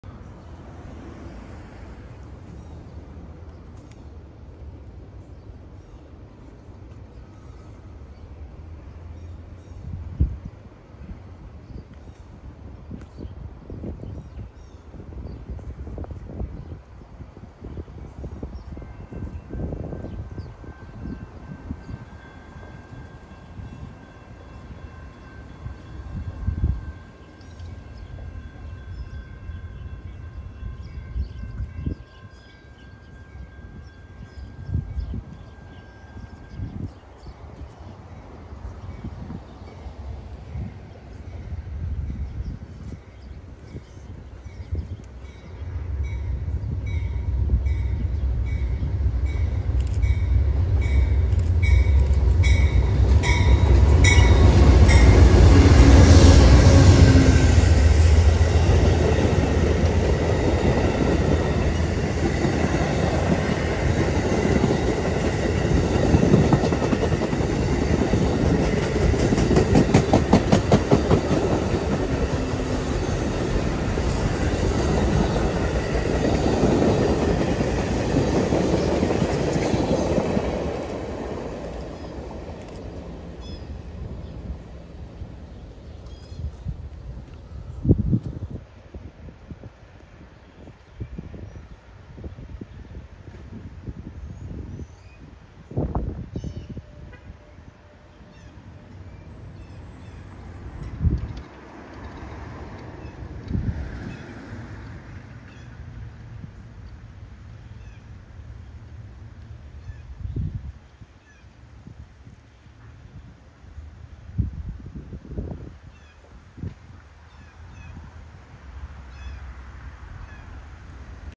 Amtrak 92 Silver Star with Two Private Coaches
9/8/23
Ashland, VA
3:00pm

Private Railcars Info

PRCX 800971   Pacific Home
VRIC 7              Kitchi Gammi club